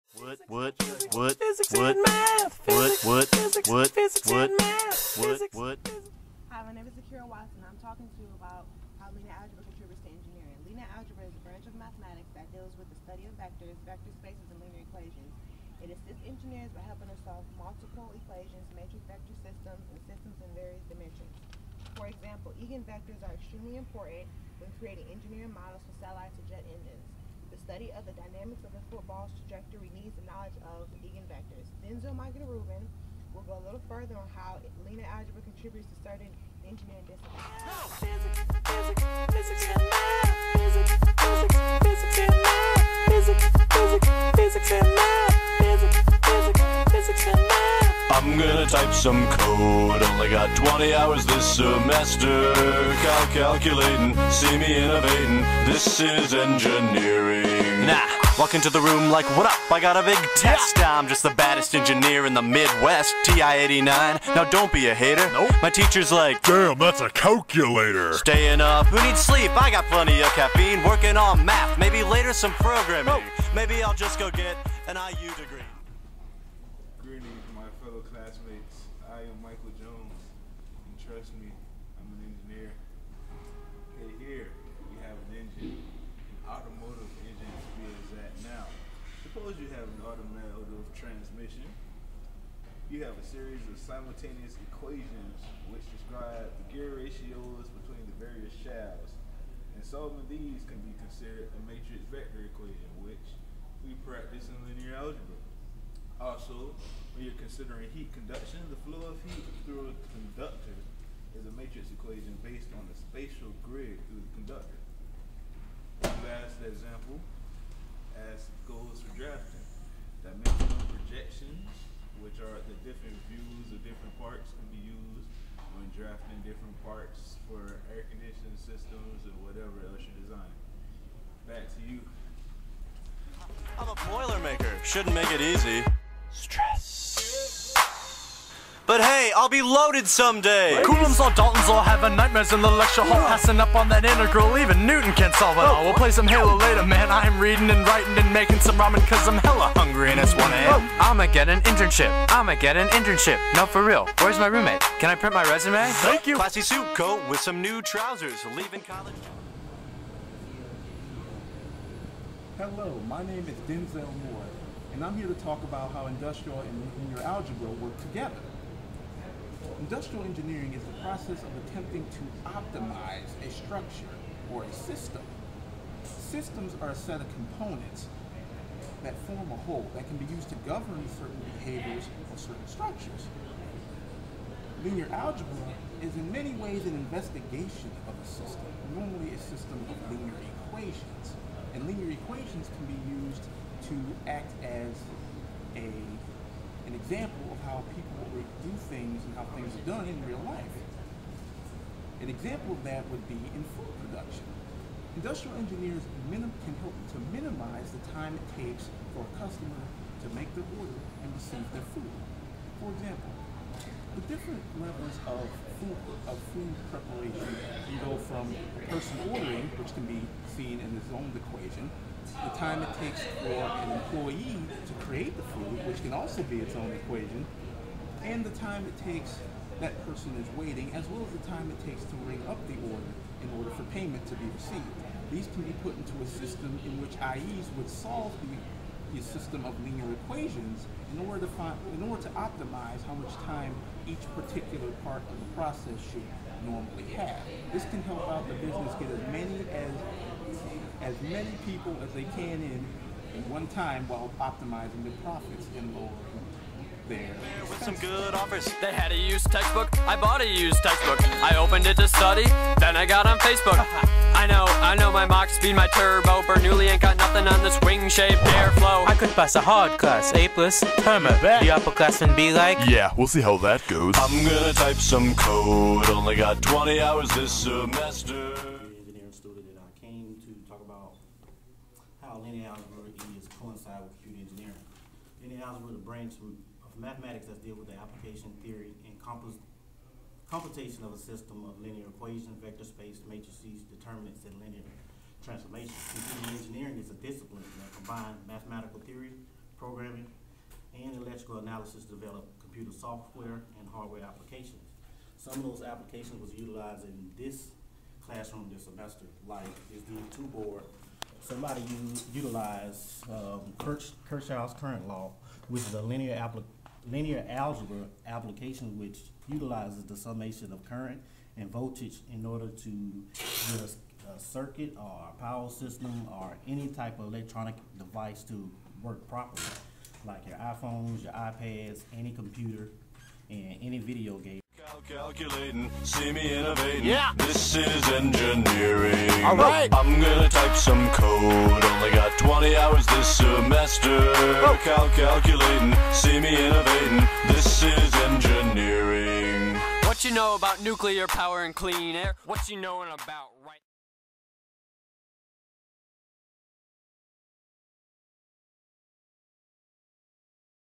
Linear Algebra Applications in Engineering
Final project for MAS3105: Linear algebra on how linear algebra is applied in Engineering